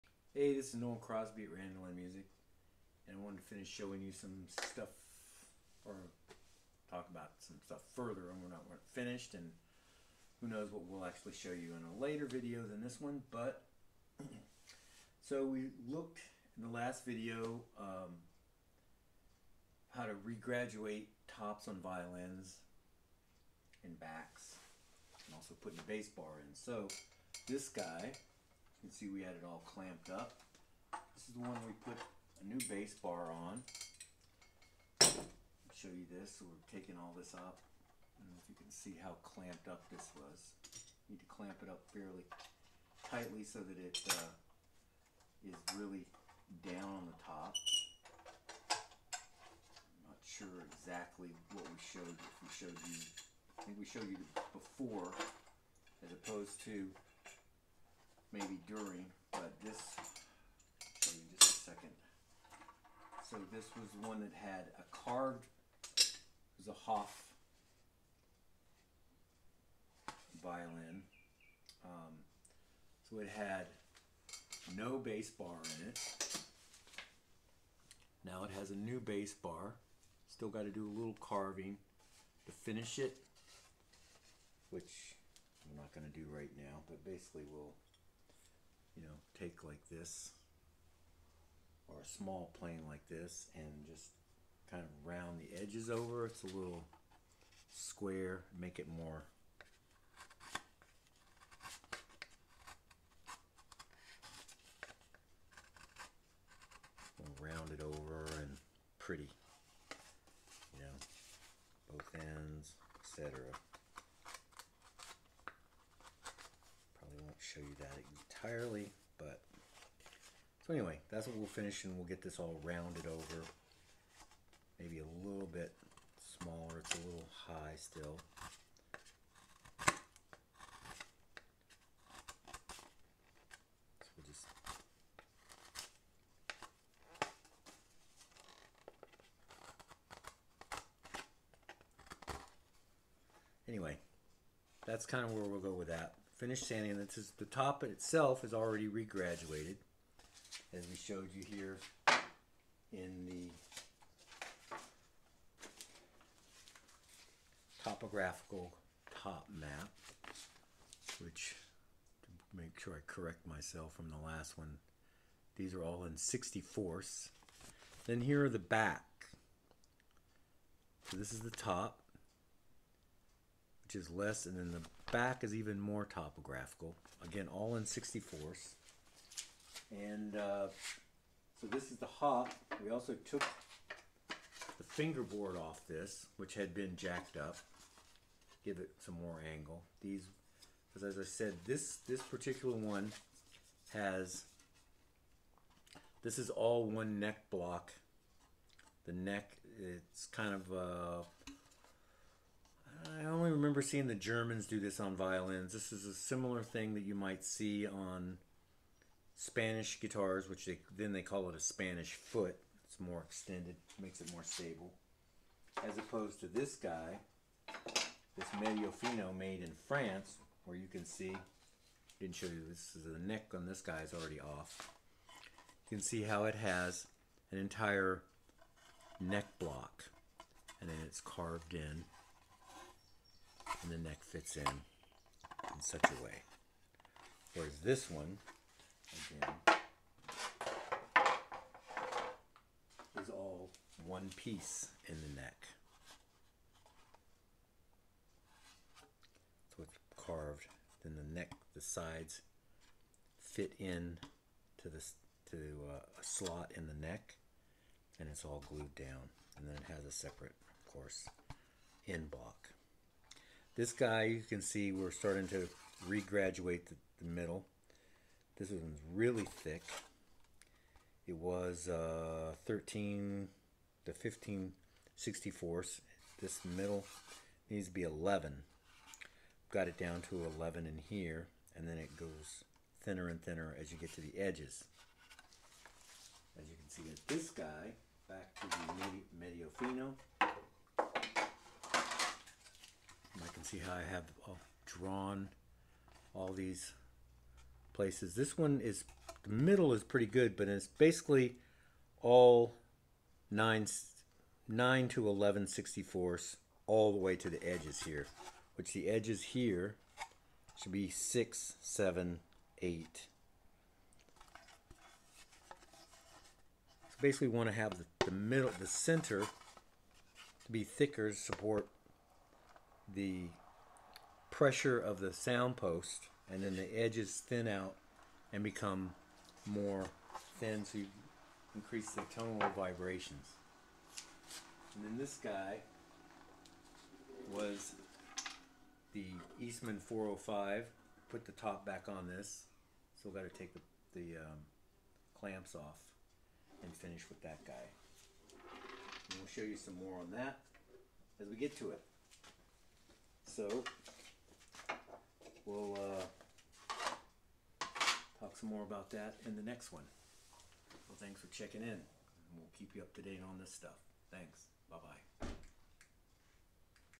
Violin Restoration, top calibration at Randolin Music Part 2
This is episode 2 showing violin restoration/top and back recalibration at Randolin Music